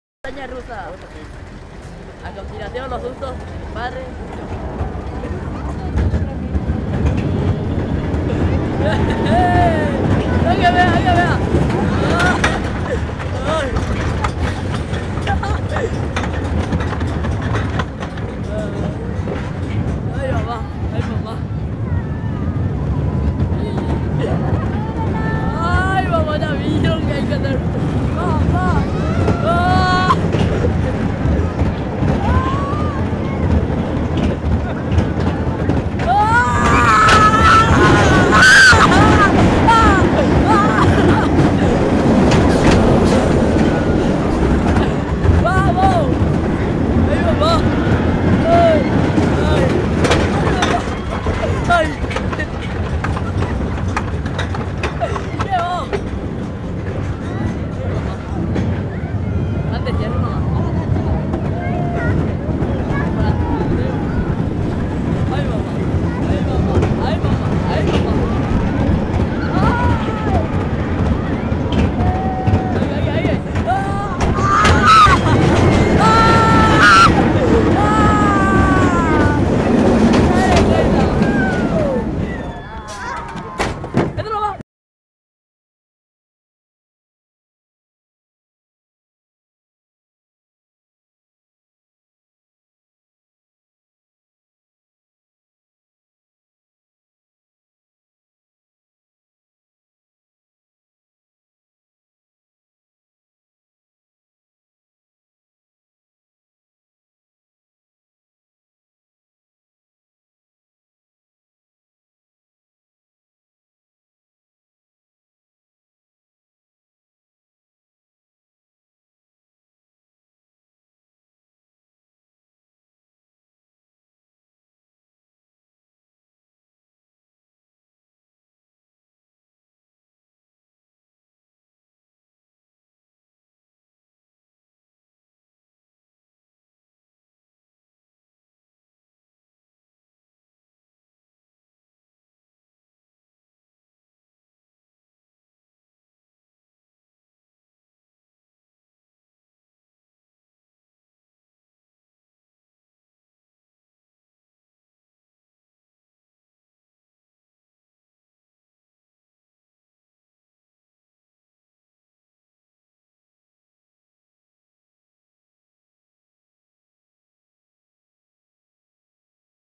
Expo Santa Rita 2010, dos vueltas en la montaña rusa
adrenalina dosificada en el recorrido de la mini montaña rusa del parque de la Expo